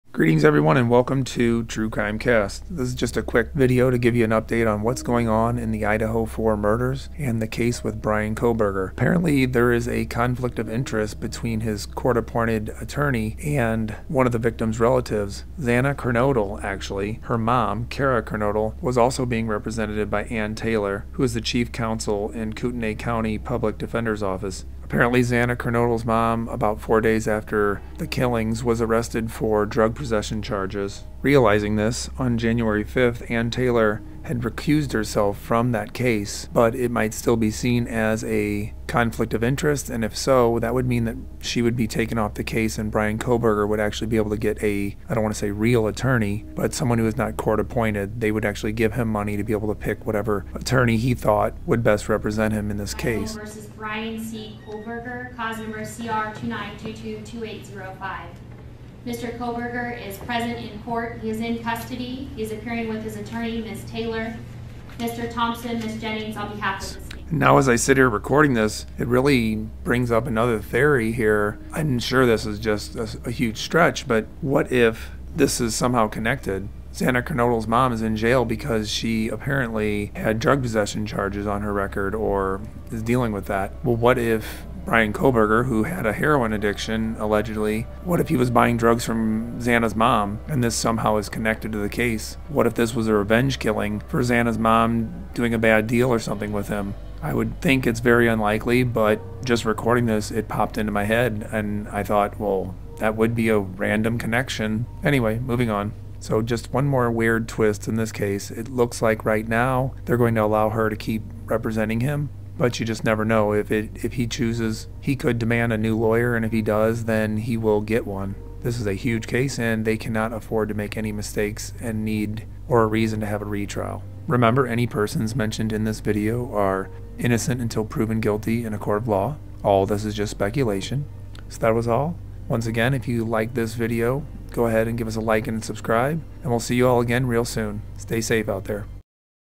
Conflict of interest? Drug connection?
Quick video on the latest events in the Idaho Four / Bryan Kohberger murder trail. There's now a conflict of interest that ties Xana's mom to this case, opening doors for other possible connections as well.